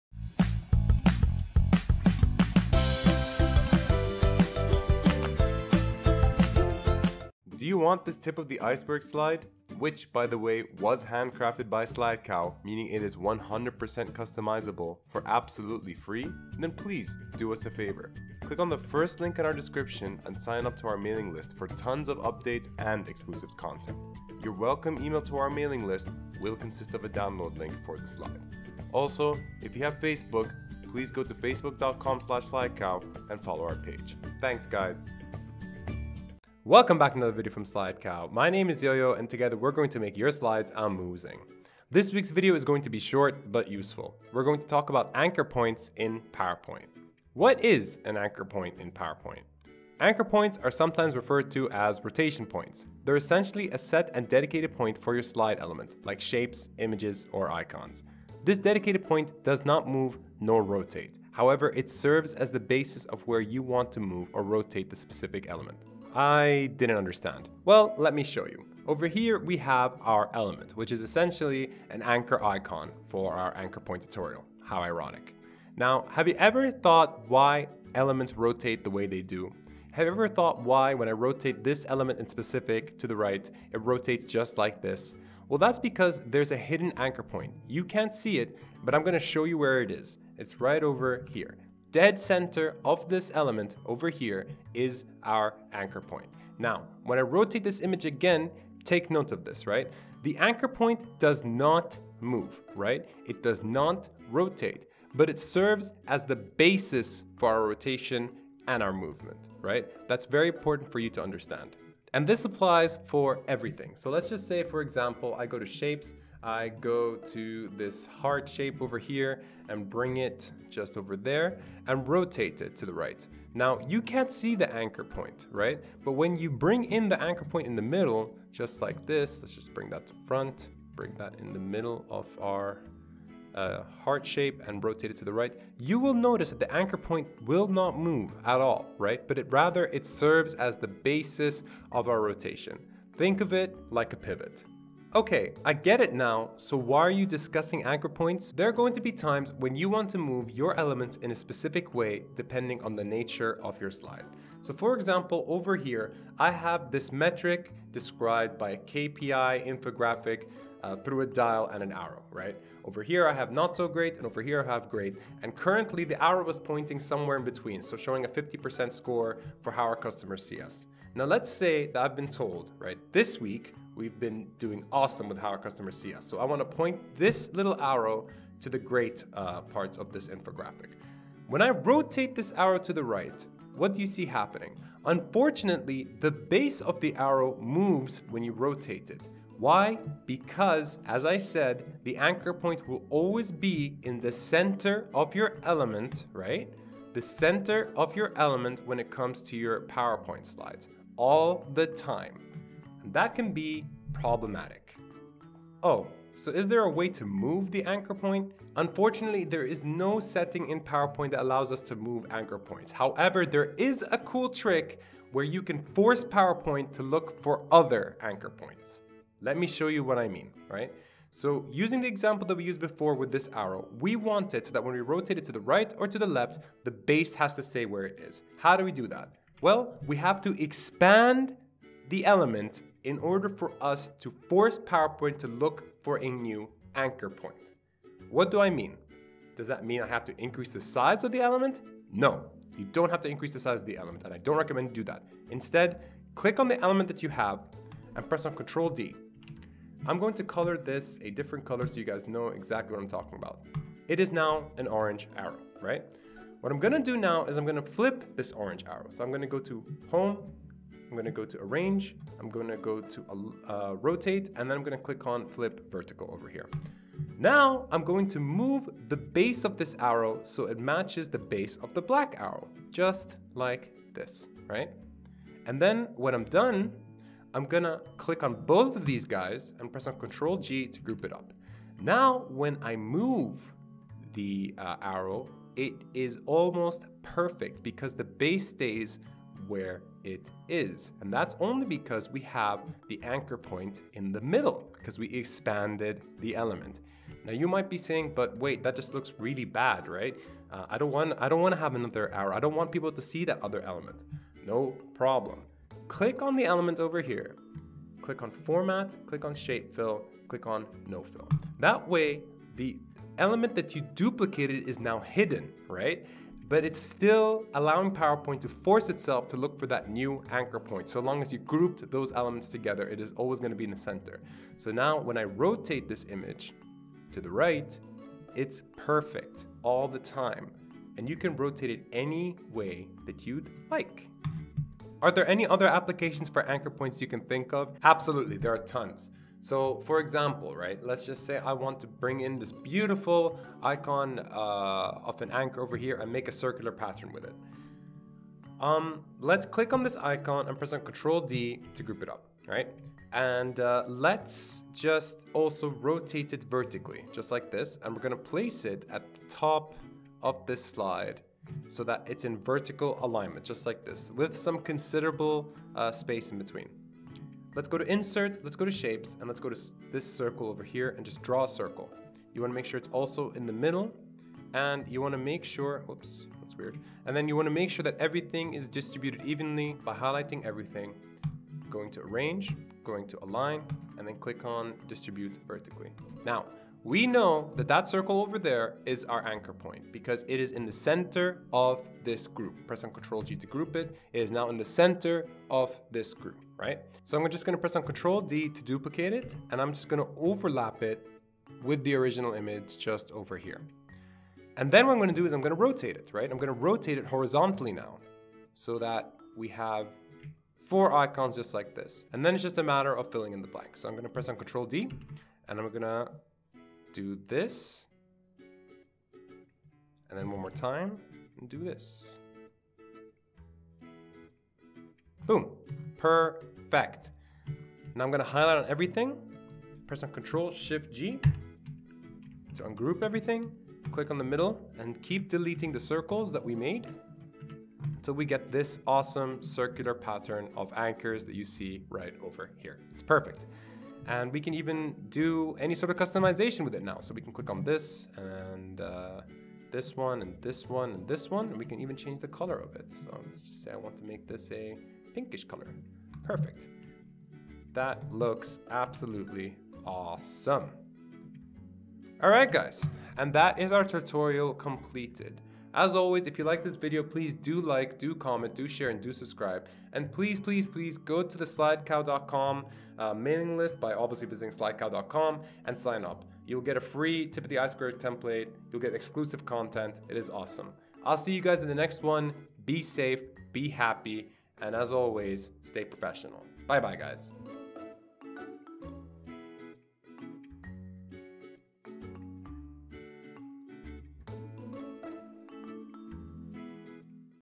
PowerPoint Tutorial: A Guide to Anchor Points and Rotation
► Looking for more tutorials?

This week's tutorial is dedicated to those PowerPoint ninjas who really want to master their rotation game. We're going to be talking about anchor points and rotation in PowerPoint.

Anchor points are sometimes referred to as rotation points. They’re essentially a set and dedicated point for your slide element (like shapes, images or icons). This dedicated point does not move, nor rotate. However, it serves as the basis of where you want to move or rotate the specific element. Think of it like a pivot.

I know it's a little confusing, but as I explain in the video, the elements on our PowerPoint slide rotate because there is a set and dedicated anchor point in the middle of that specific element.

The way PowerPoint sets its anchor points can be problematic. If we want our shape to be rotated in a very specific way so that the bottom part doesn't move, but the top part does, there is no setting on PowerPoint that can let us do that.

BUT, there is a cool trick you can do to force PowerPoint to look for another anchor point.

The trick is to expand the element and to force the anchor point to move into the middle. Do I mean you should increase the size of the element?

So what do I mean?

Check out the video to find out.

You should consider this technique when you:

- Have a specific PowerPoint element that you want rotated in a specific way (e.g. an arrow where you want to move only the arrow head and not its base).

- Create a circular pattern for a set of elements on your PowerPoint slide.

This PowerPoint tutorial will teach us how to:

- Identify the anchor points for our PowerPoint elements.
Learn how to expand elements to force PowerPoint to look for a new anchor point in order to ensure that the element rotates it the way we want it to.

- Learn how to create circular patterns with the technique of identifying and expanding anchor points.